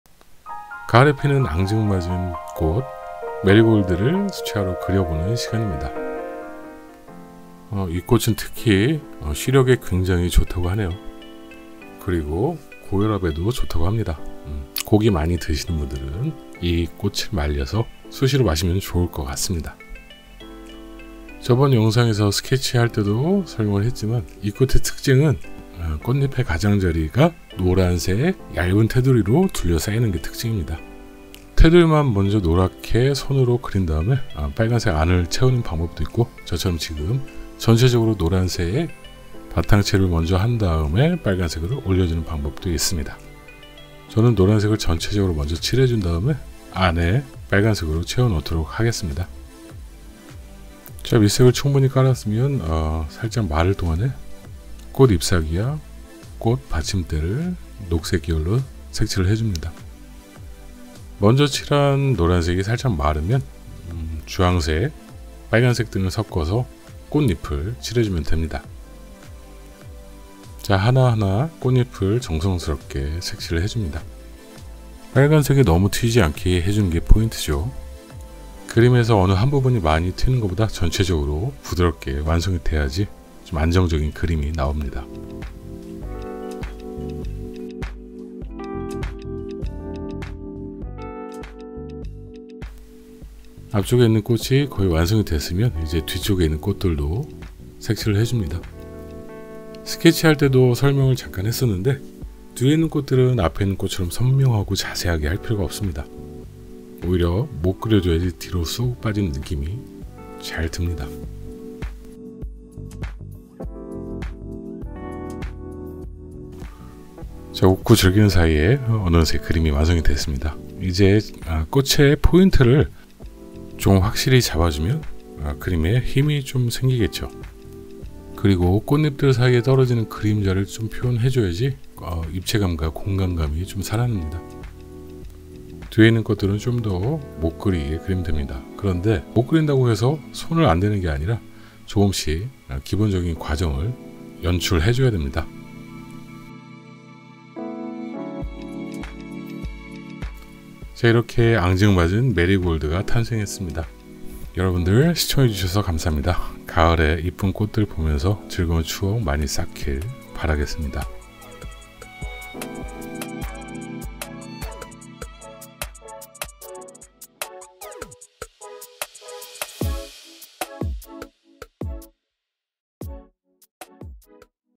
가을의 앙증맞은 꽃 메리골드/ Marigold water color
메리골드꽃을 수채화로 그려보는 영상입니다